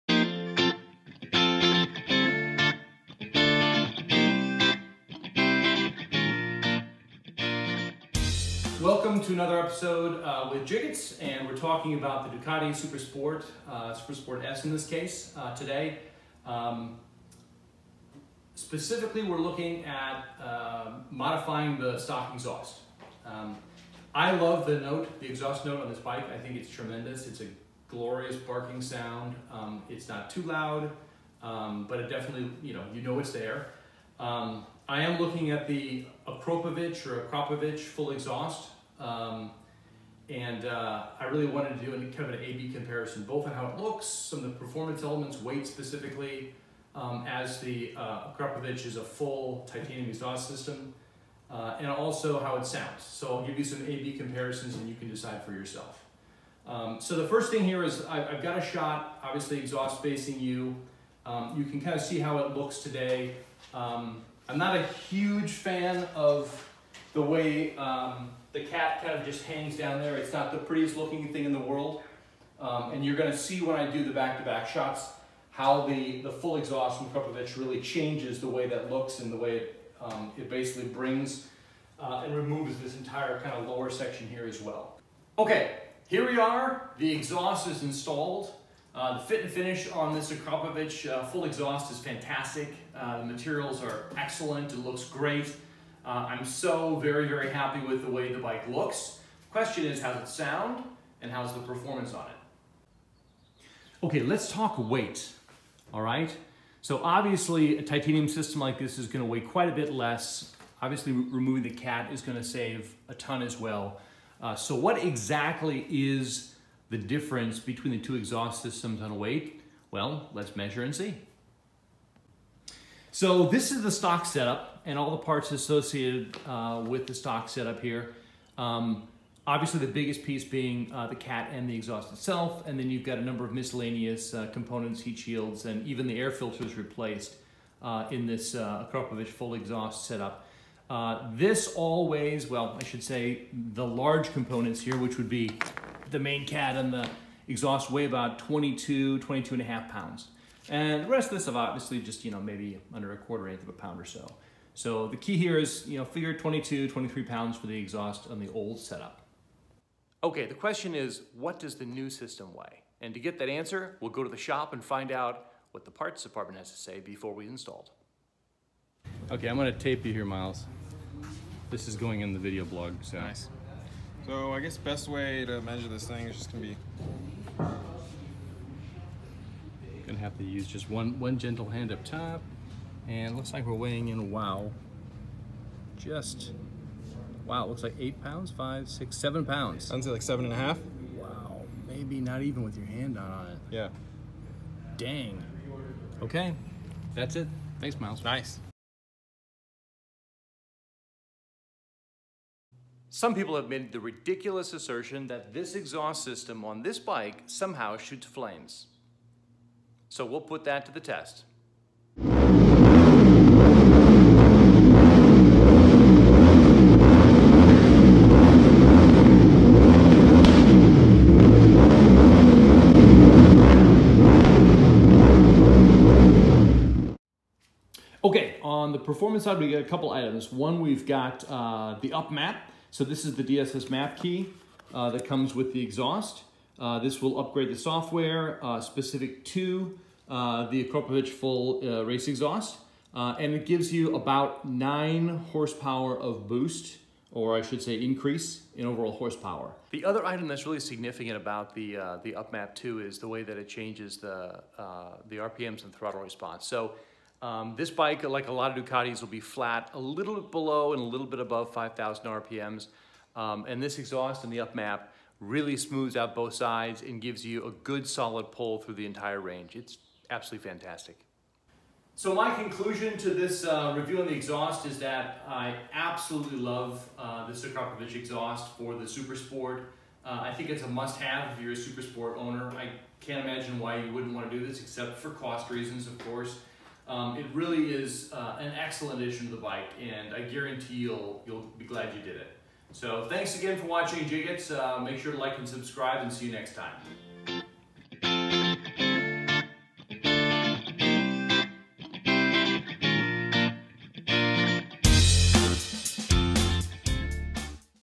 Akrapovič Full Exhaust Review
Today I review the Akrapovič Full Exhaust for the Ducati Supersport.

Price: $2,900 (ouch!)

Please tell me all about how I mispronounce Akrapovič.